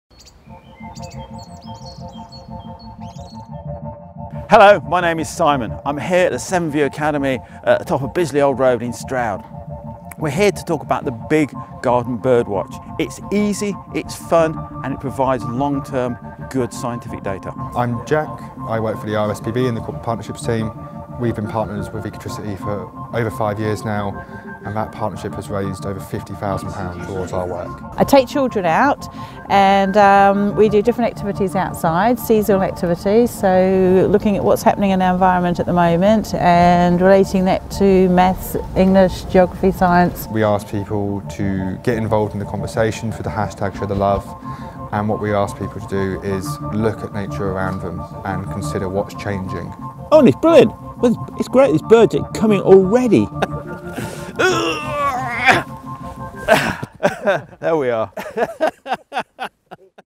Looking back at nature month.
We've had a great month focusing on nature. We've made bird boxes, got involved in some birdwatching  and also started to think about how to prep the garden for spring.

A big thanks to the RSPB for being our partner of the month. We look forward to continuing our work together in partnership in the future!